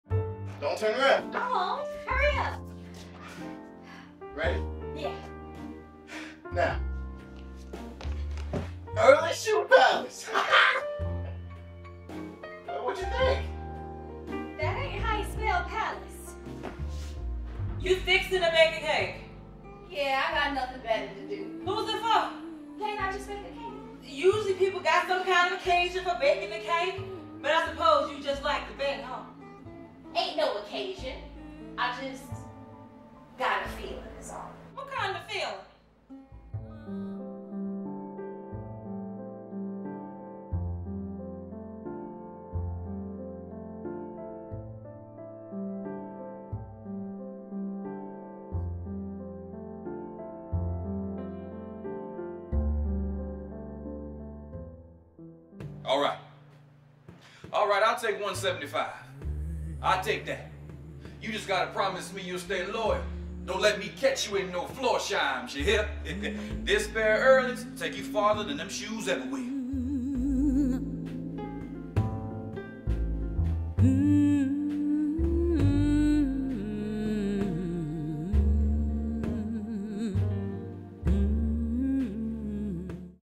Raven Theatre Presents: "The Last Pair of Earlies" by Joshua Allen
Raven Theatre brings back live performances to Chicago with the world premiere of "The Last Pair of Earlies" by Joshua Allen, directed by Wardell Julius Clark. This Chicago tale is filled with laughter, tears, dreams, and the hard truths about growing up and growing apart.